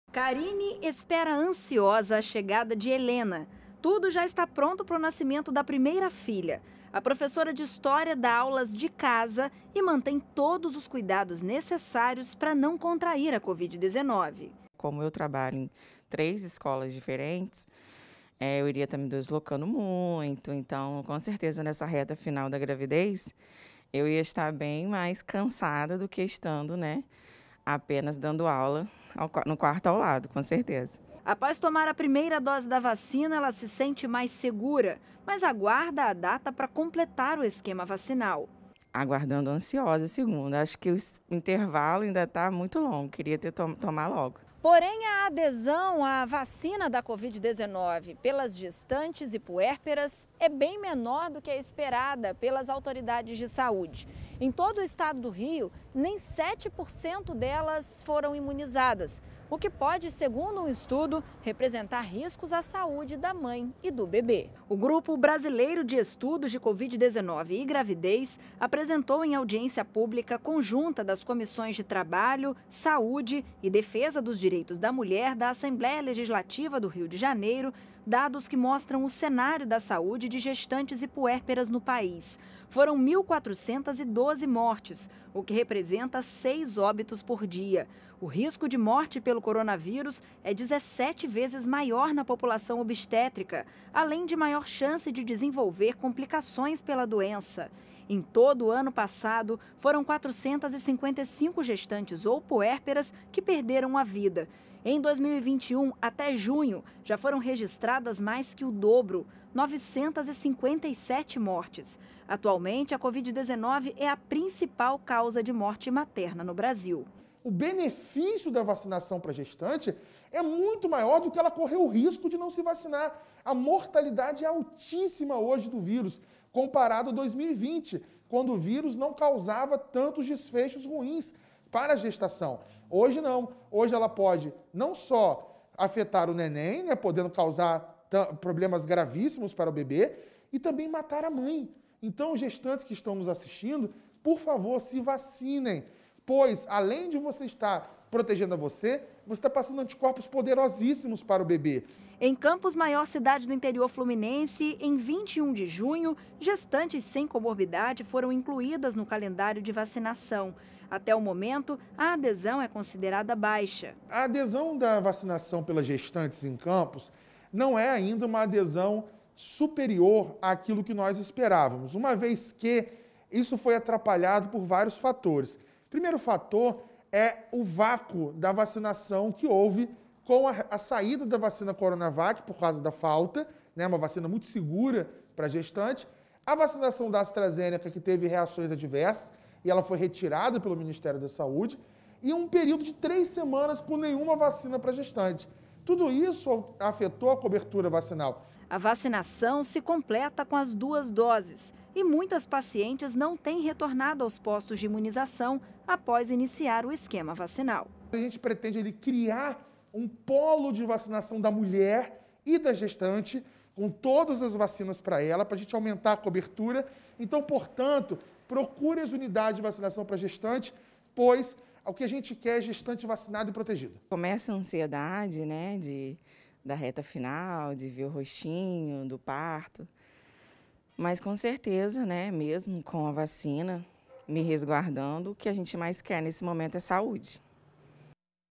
Importância da imunização completa para gestantes
Seis gestantes perdem a vida todos os dias no Brasil por causa da covid-19. Os dados foram apresentados em audiência pública na Alerj. Os casos de mortes reforçam a necessidade da vacinação para grávidas e puérperas.